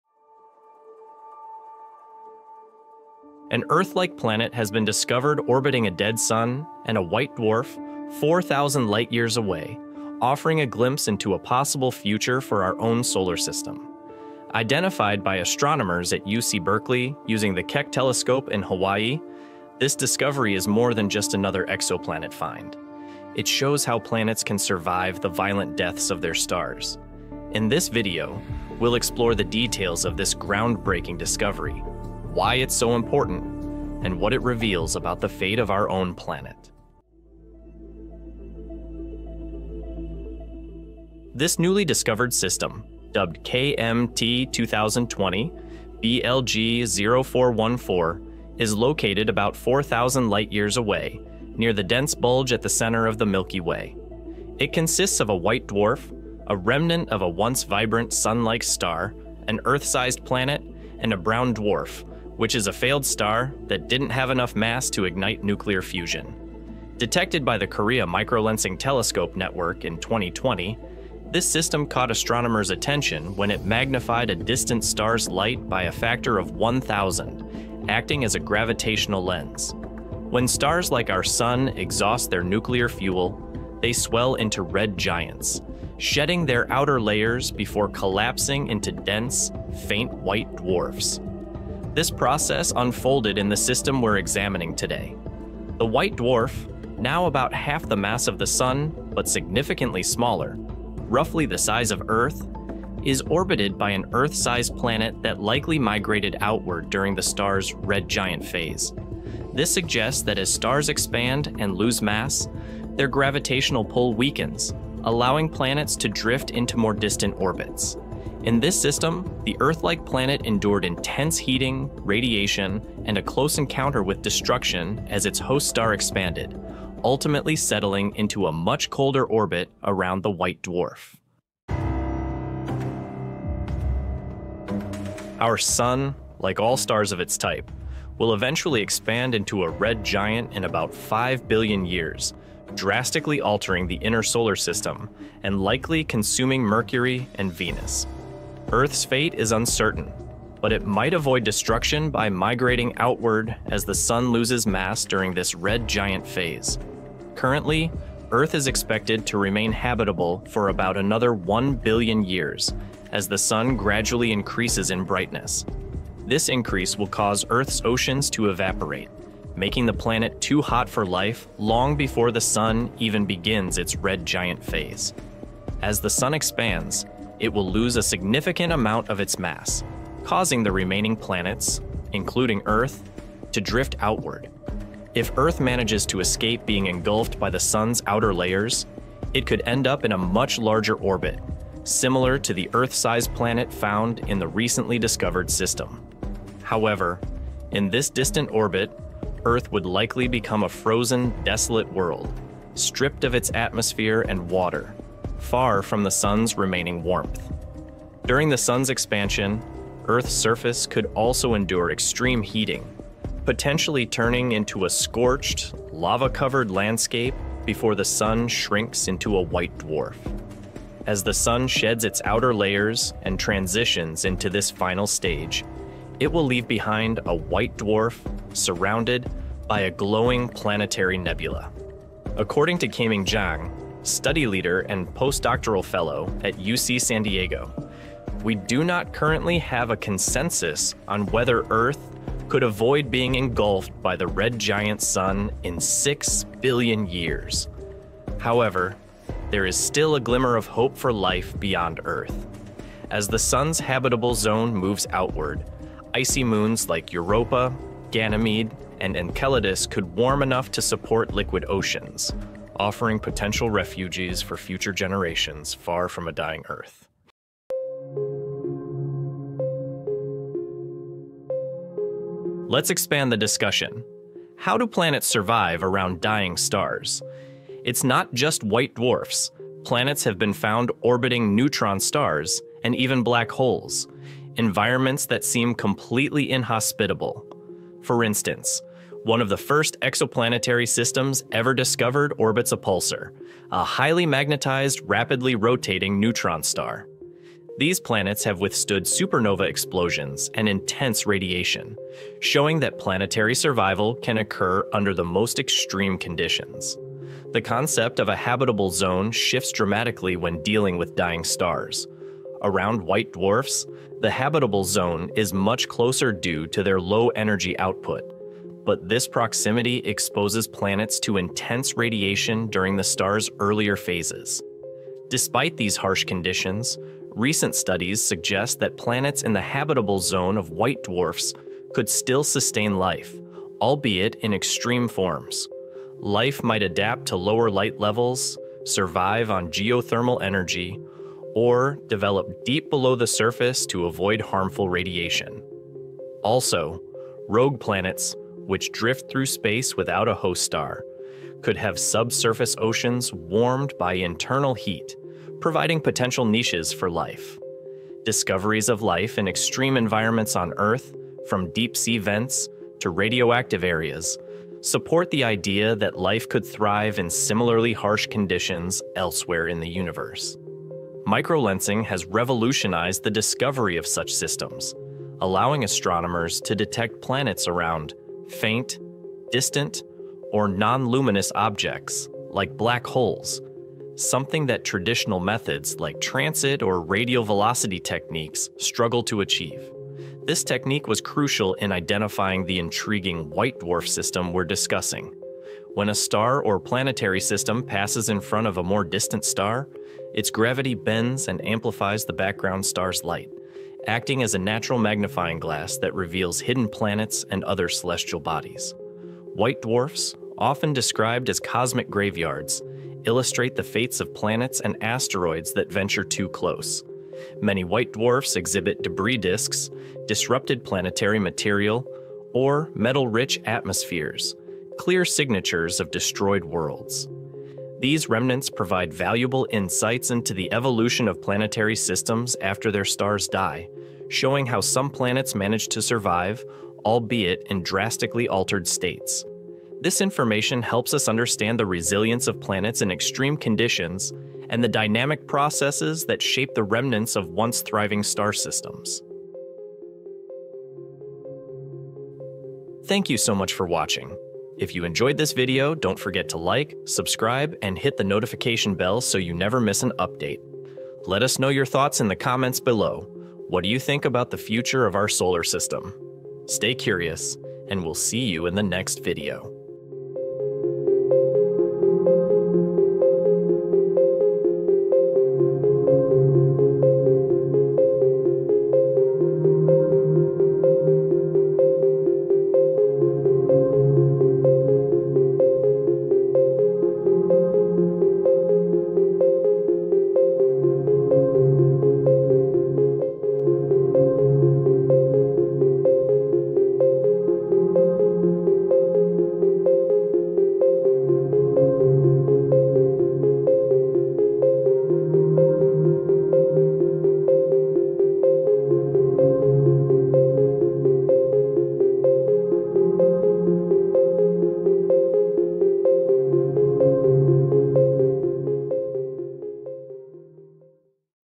Mirror Image! Discovery of an Earth-Like Planet Around a Dead Sun Sparks New Hope
Discover the potential fate of Earth as we explore a recently found Earth-like planet orbiting a dead star, or white dwarf. This glimpse into the distant future shows how planets might survive the death of their stars but in drastically altered states. Learn how the Sun’s evolution could transform our solar system, and what might happen to Earth in the end. Will our planet endure, or is destruction inevitable?

Chapters:
00:00 Introduction
00:37 The Discovery and Its Implications
02:11 What This Means for Earth’s Future
04:27 Planetary Survival Around Dying Stars and Extreme Environments
07:32 Outro
07:50 Enjoy